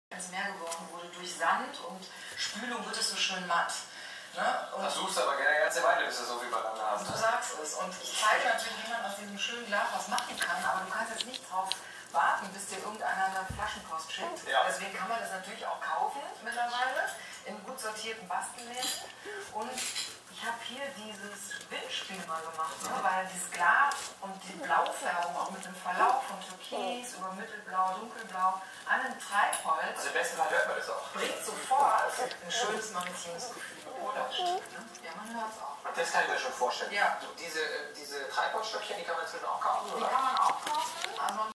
Beaglewelpen A1 Wurf 13 Tage
Jetzt kommt langsam Bewegung in die lustige Truppe. Man kann stundenlang zusehen.